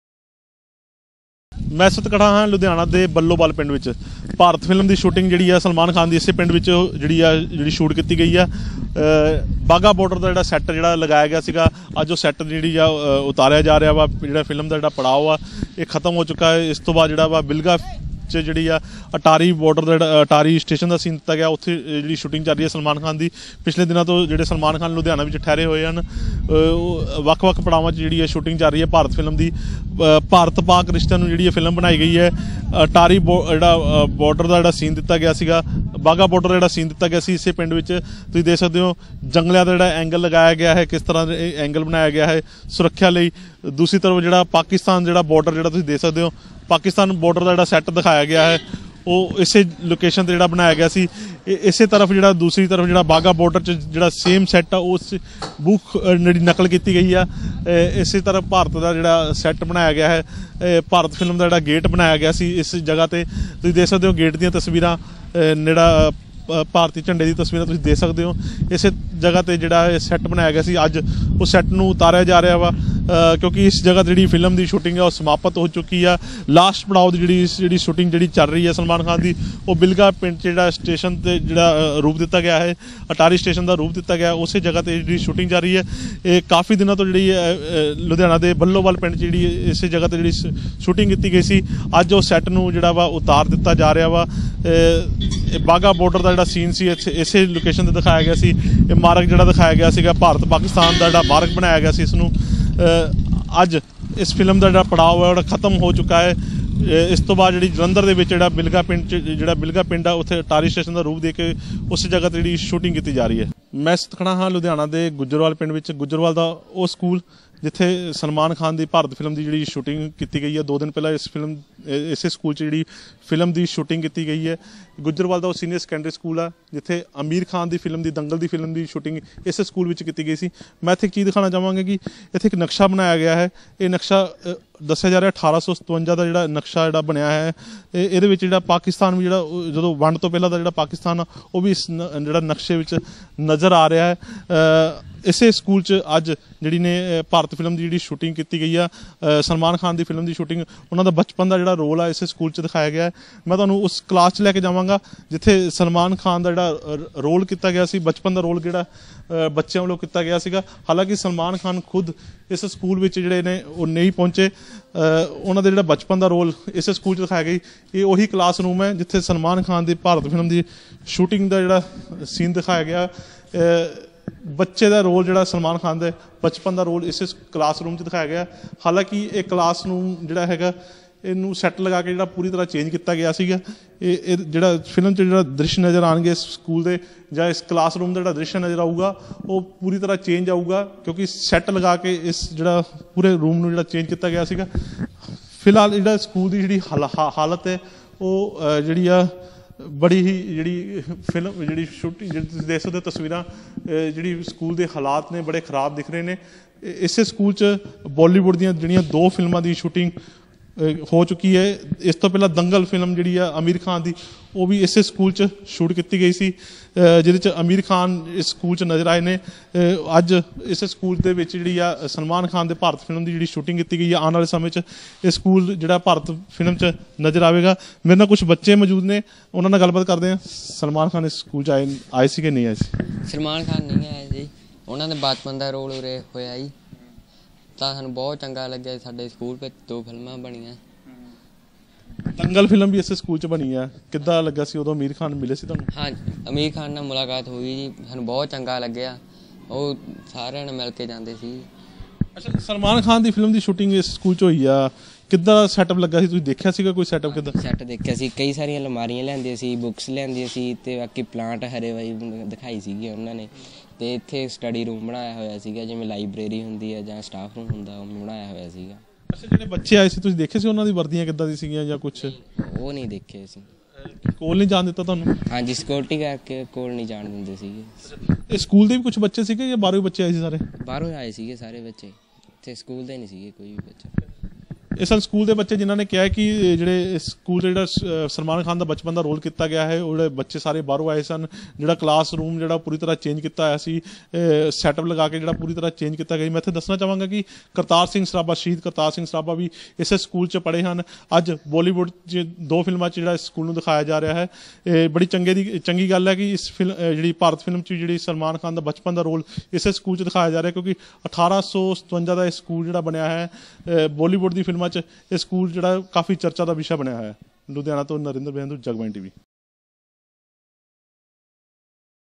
Exclusive! from sets of 'Bharat' film
Balowal & Gujarwal locations were selected for the film
Govt. Sr. Sec School was also taken in film 'Dangal'
Salman's childhood character has given shots in school
Wagha Border was created in Ludhiana's Balowal